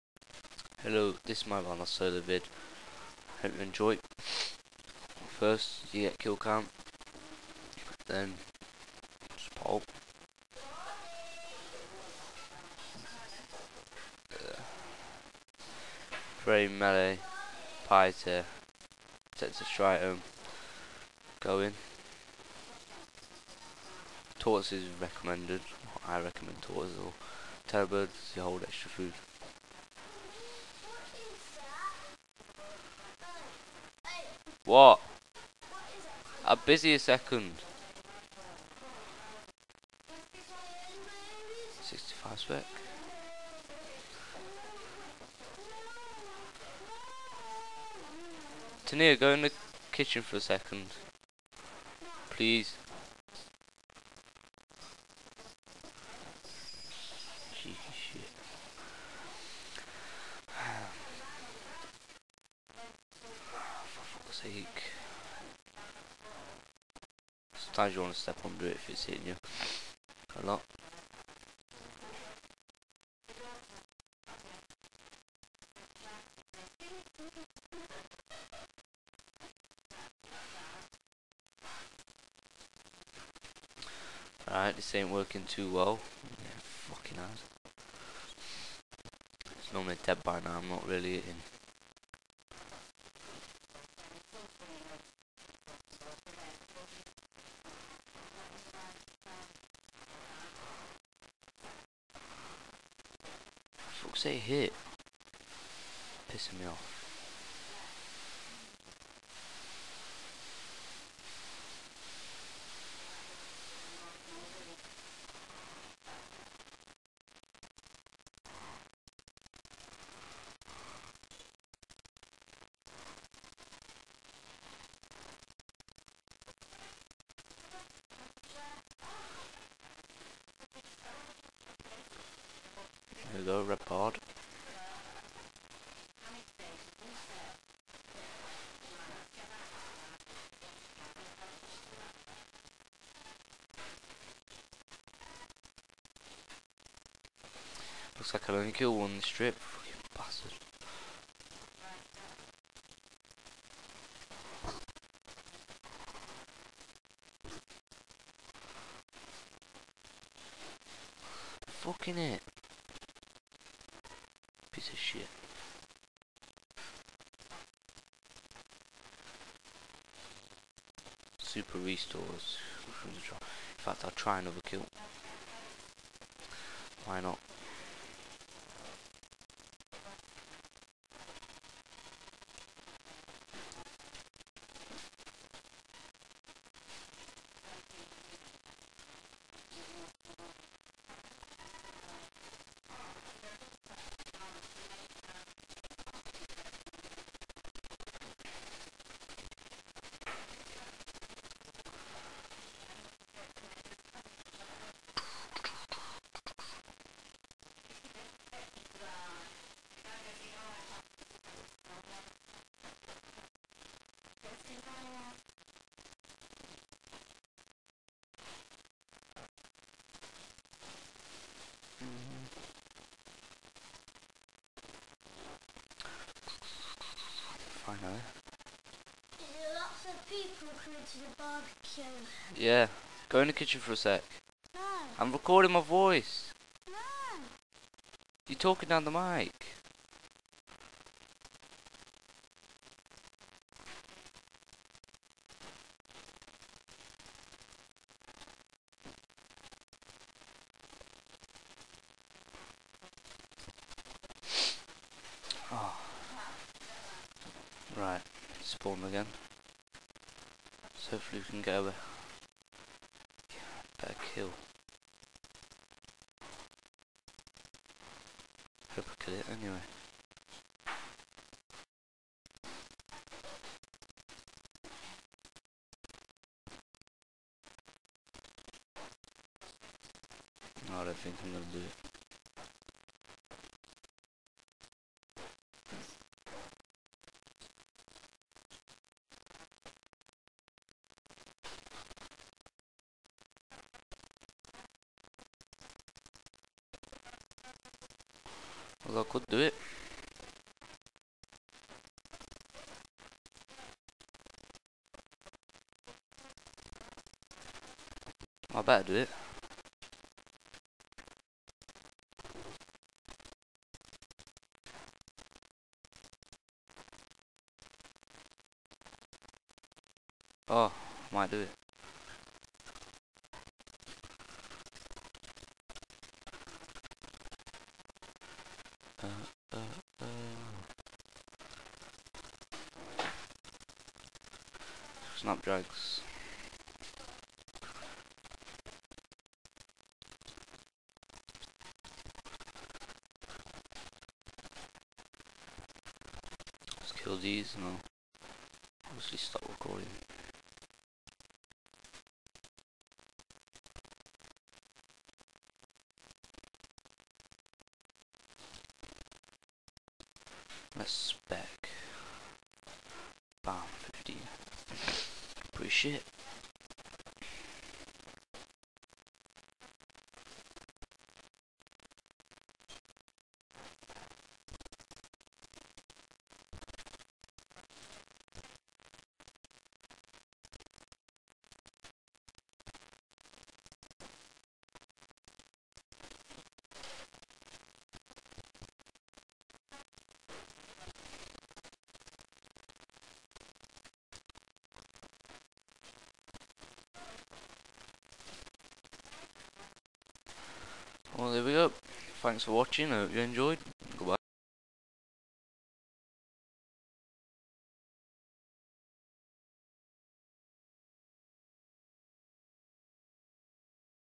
bady021's bandos solo commentery
hey hope u enjoy commentery vids cus i got 1 of me soloing bandos =] hope u enjoy